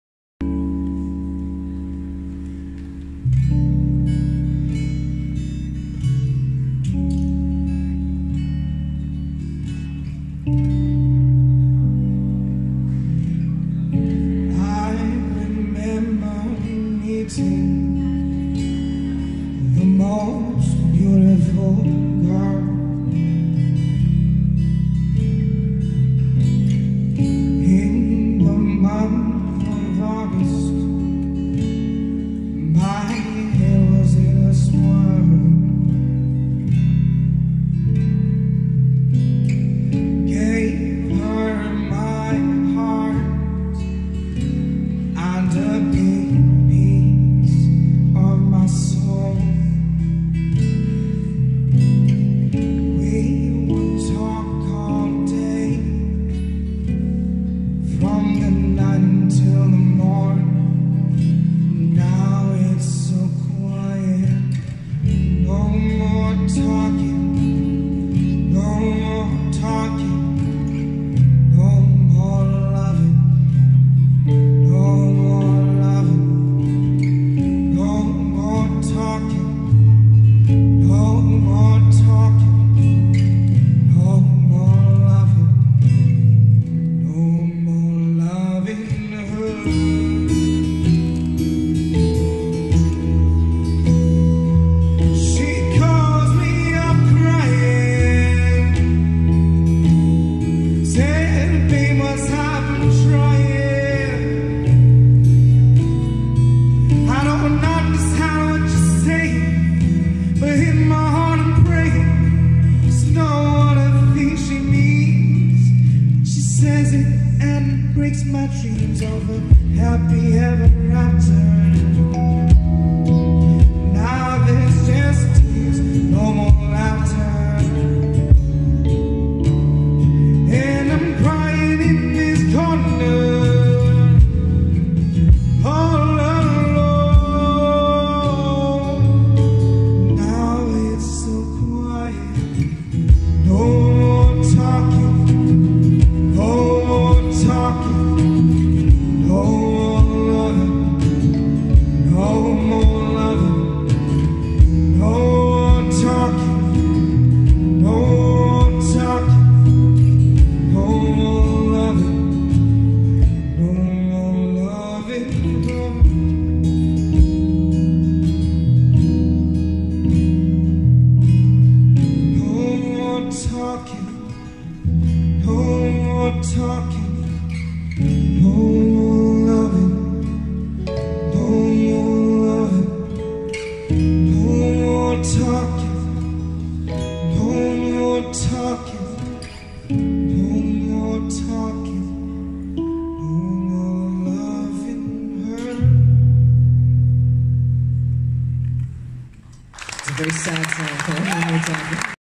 Daniel Sid
Performing at the Esplanade Concourse ALL THINGS NEW Singaporean music artists embark brand new chapeaters of their musical journays